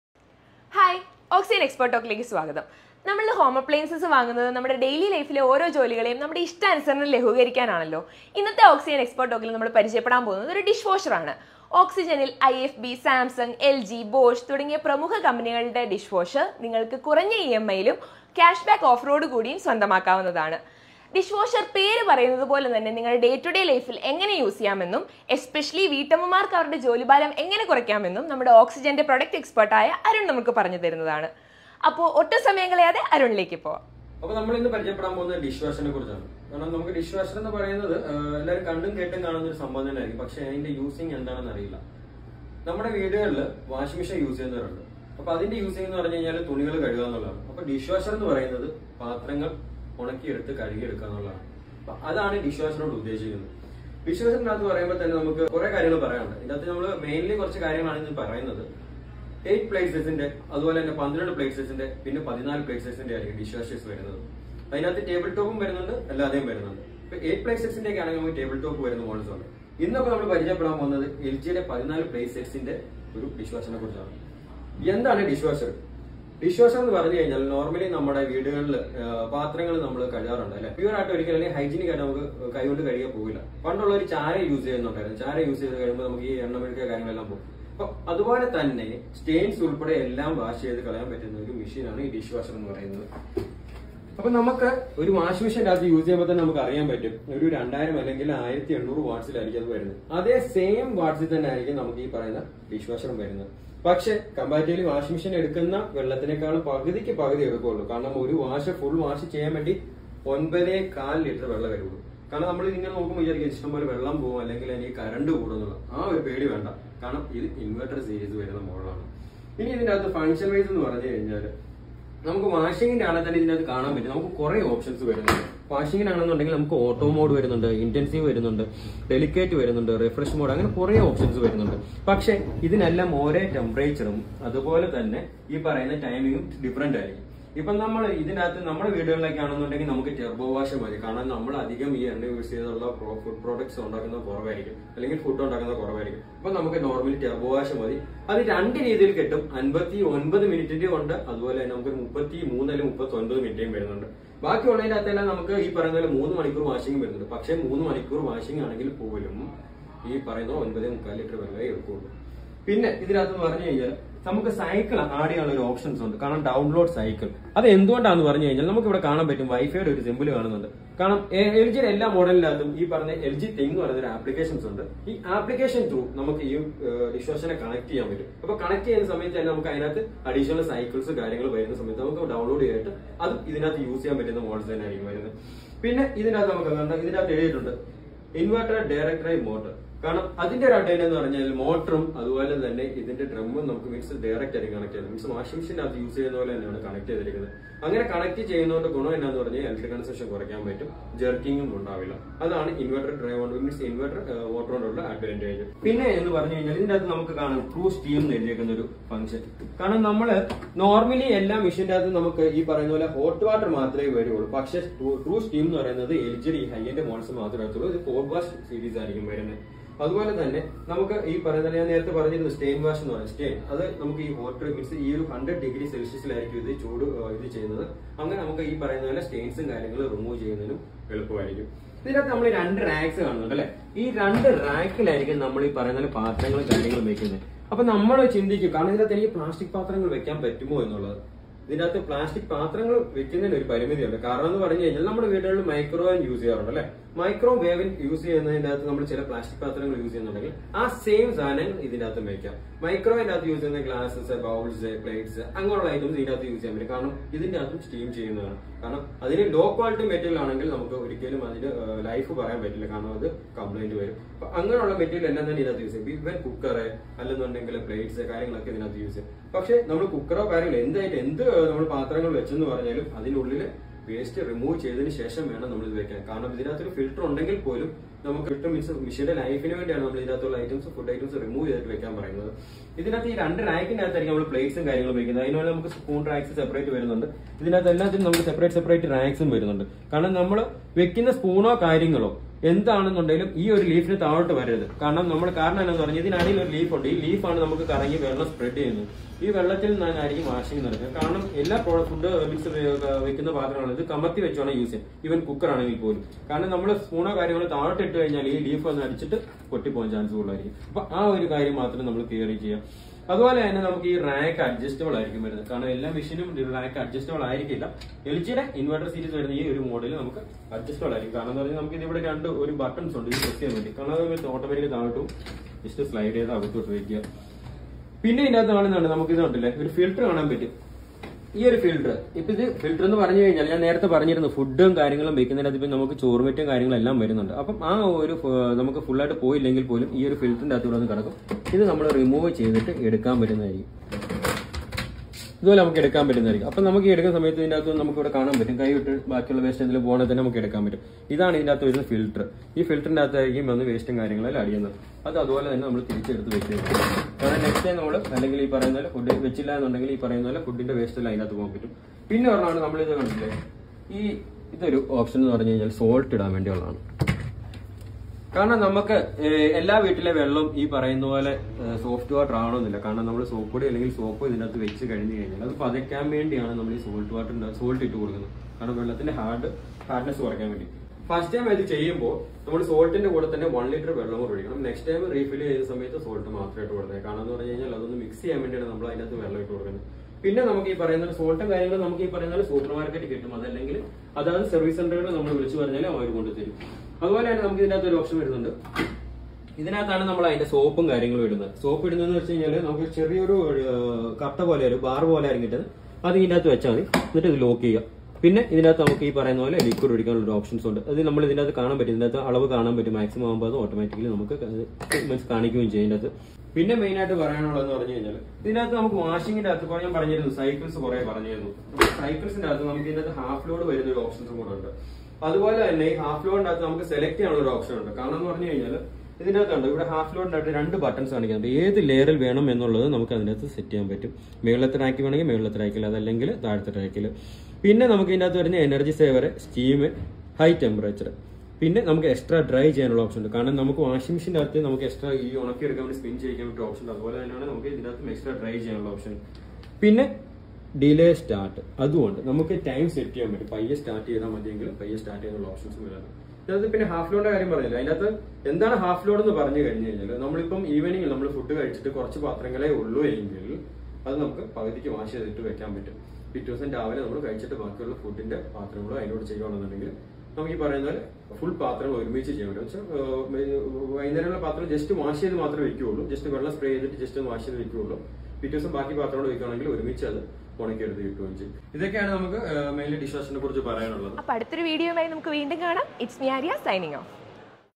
Oxygen Expert Talk! | Dishwasher
Oxygen Expert Talk!
കേരളത്തിലെ ഏറ്റവും വലിയ ഇലക്ട്രോണിക്സ്, ഡിജിറ്റൽ ഗാഡ്ജറ്റ്സ്, ഹോം അപ്പ്ളൈൻസസ്‌ ഷോറൂം ശൃംഖലയായ ഓക്സിജൻ അവതരിപ്പുക്കുന്നു വിവിധ ബ്രാൻഡിൽ ഉള്ള ഡിഷ് വാഷറുകൾ! ഉടൻതന്നെ ഓക്സിജൻ സന്ദർശിക്കൂ ജോലികൾ ഇനി expert ആയി ചെയ്യൂ.